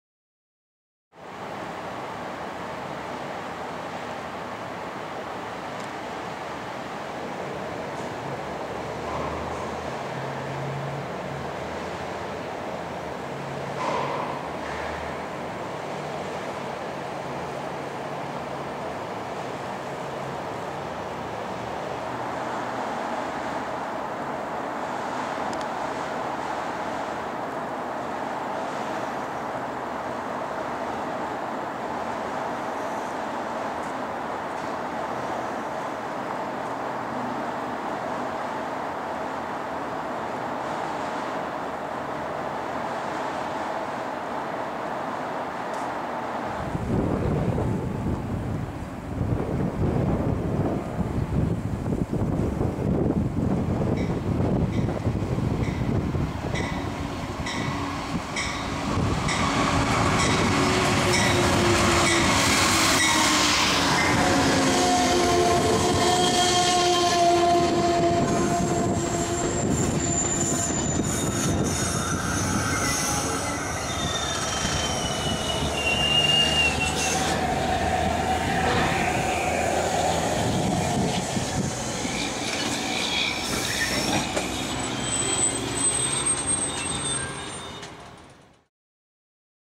Amtrak 335 Arrives Sturtevant (Ext. Scene) - 11/19/2011
November 19, 2011 (Extended Scene): During a points run day, Amtrak 335 approaches and arrives Sturtevant from Chicago and Glenview. From the 3rd level landing in east foot bridge tower, the train can first be seen 7 minutes before it's arrival at the station.